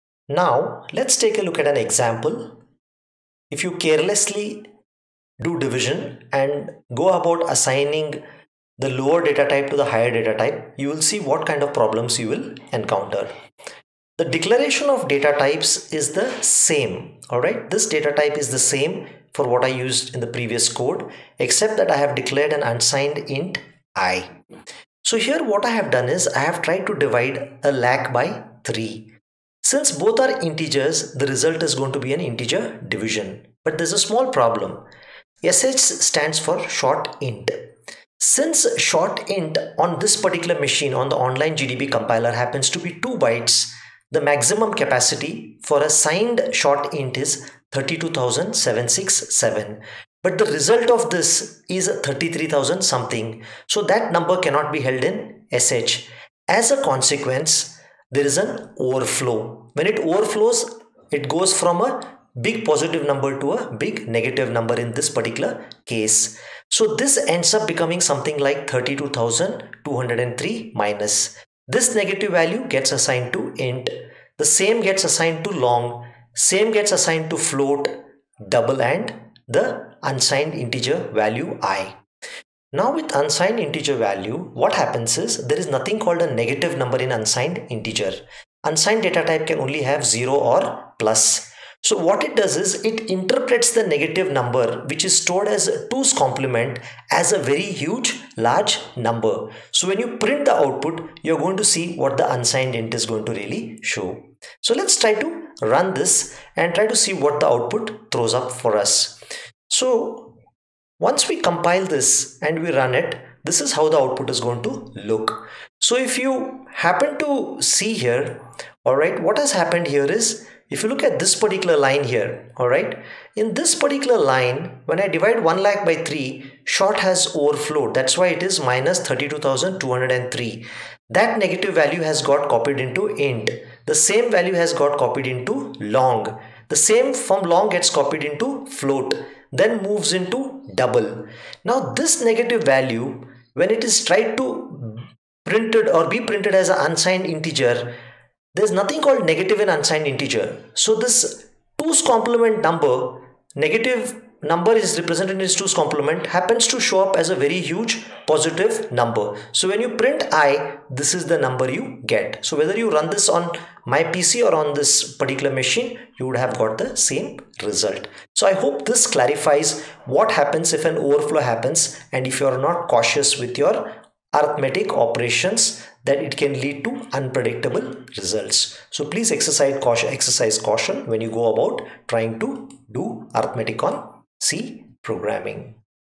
Data Types Concept Code 3
Division Assignment among basic data types
// Overflow
// EXAMPLE 3
NOTE: INCLUDE THE HEADER FILE stdio.h
int main()
    short int sh ; long int lg ; unsigned int i;
    int it ;     float ft ;     double db ;

    i= db = ft = lg = it = sh = 100000/3;

    printf ("short = %d,int = %d ,long = %d \n", sh, it, lg);
    printf ("float = %6.2f, double = %6.3f\n", ft, db);
    printf ("Unsigned int i = %u \n", i);
     /* s = -32203    i = -32203  l = -32203
       f = -32203.00 d = -32203.00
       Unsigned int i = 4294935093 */
    return 0;